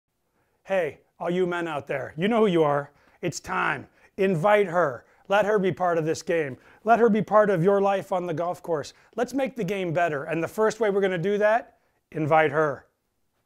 LPGA Commissioner Mike Whan Encourages Men to Invite Women to Golf
LPGA Commissioner Mike Whan encourages men to invite the women in their lives to join them on the golf course.